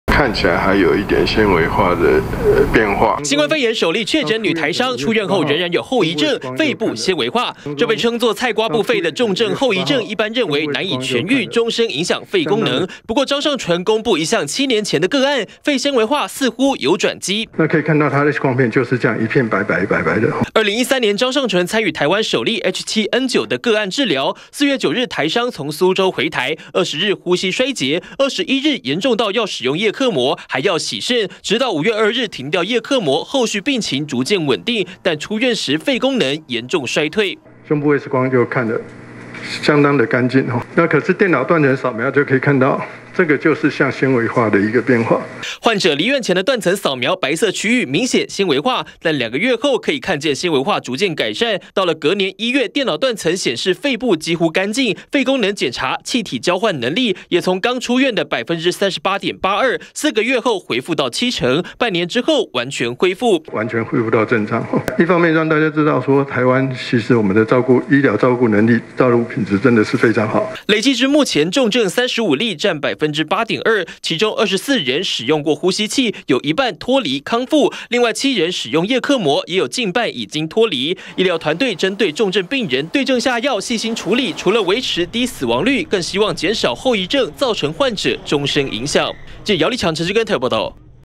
20200502中天新聞 染疫「肺纖維化」不能復原？張上淳揭「痊癒」病例
新冠肺炎沒有及時治療，可能導致病情惡化成重症。國內確診首例女台商，就出現過肺部纖維化等嚴重的症狀，不過疫情中心專家張上淳說，肺纖維化並非完全不可治癒，他就舉出7年前，他參與治療的H7N9個案，一度使用葉克膜，出院後也有炎重肺纖維化，但在台灣醫療團隊的照顧下，半年後，肺功能完全恢復。